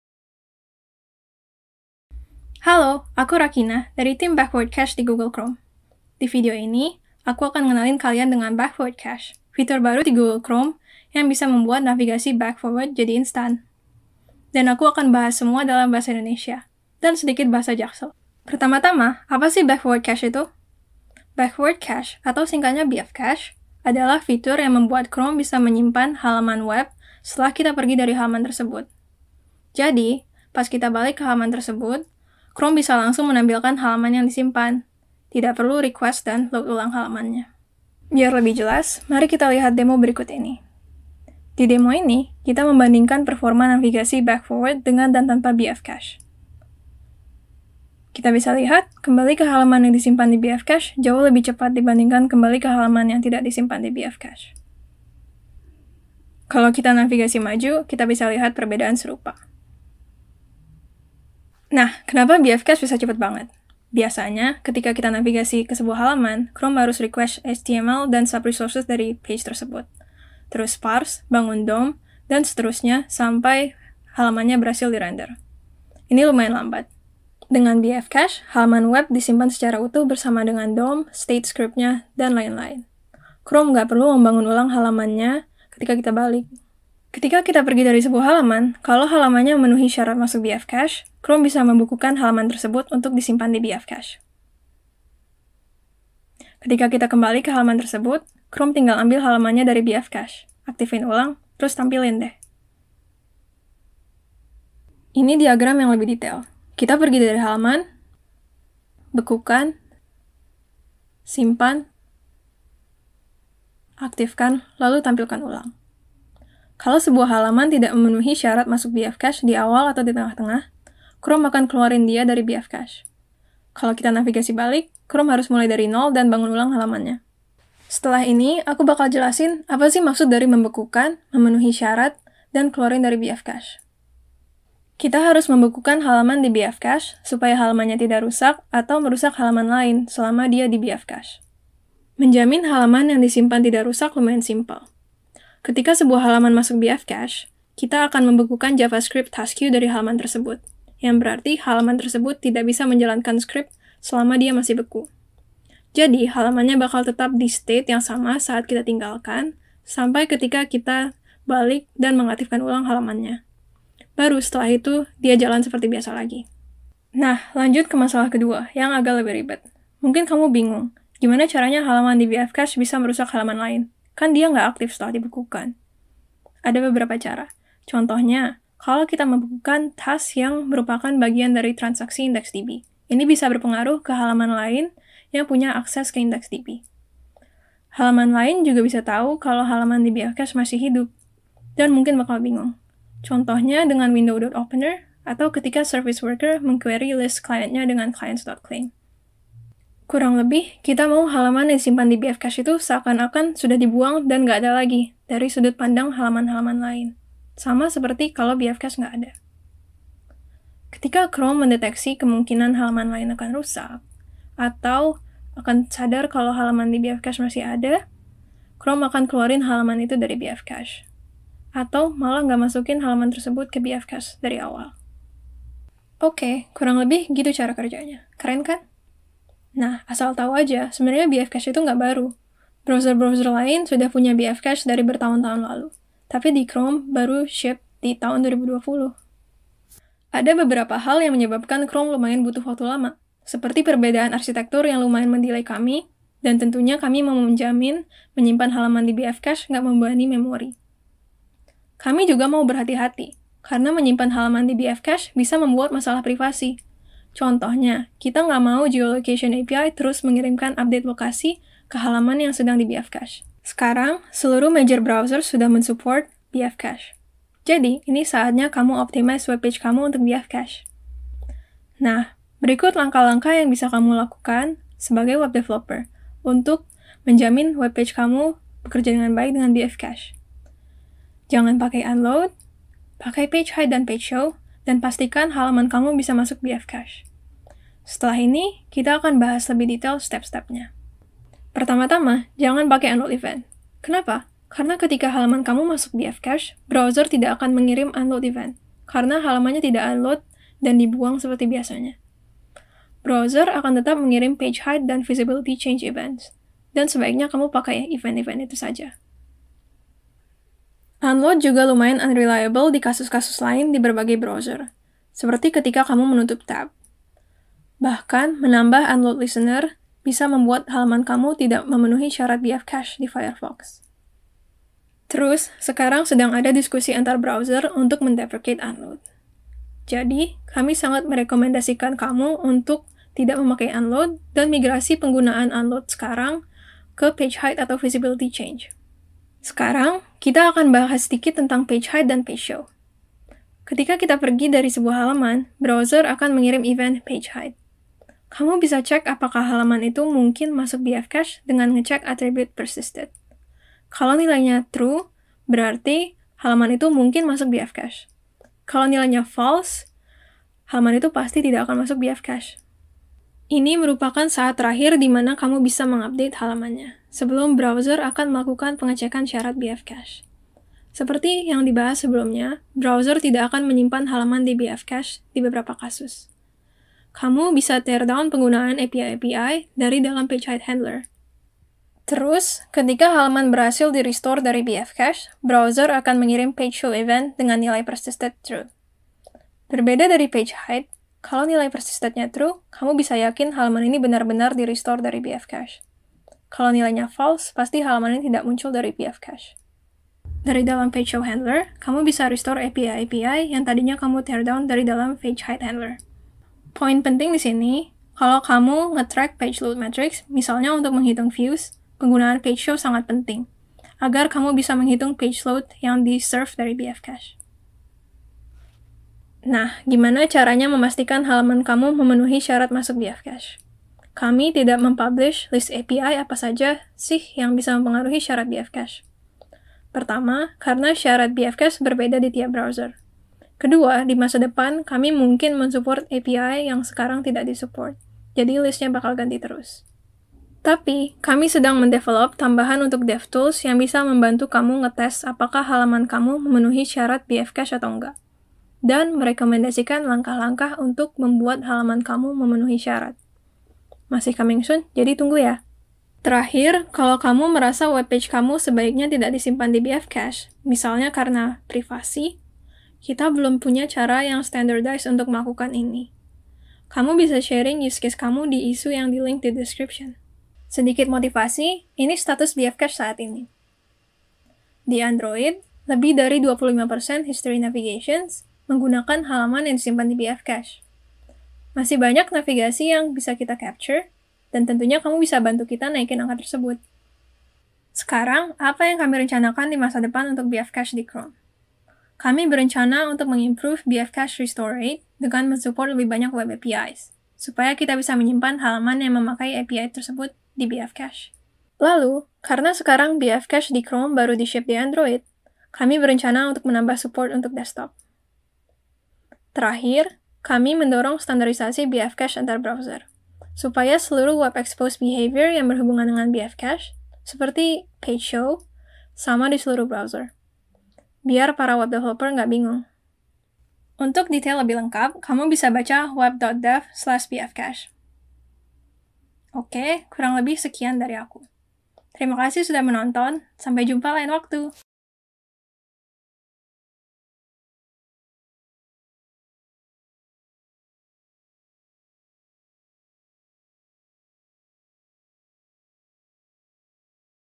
Cache de voltar/avançar (BFCache) [LEGENDADO] - Rakina Zata Amni, Google Chrome Developers
Cache de voltar/avançar (ou BFcache) é uma otimização do navegador que permite a navegação instantânea para frente e para trás. Ele melhora significativamente a experiência de navegação dos usuários - especialmente aqueles com redes ou dispositivos mais lentos. Este vídeo explica o que é, como pode afetar suas páginas da web e como garantir que suas páginas sejam otimizadas para isso. Este vídeo está em Bahasa Indonésia com legendas em português.

Palestrante: Rakina Zata Amni

FONTE:
Back/forward cache (Indonesian with English subtitles)
Rakina Zata Amni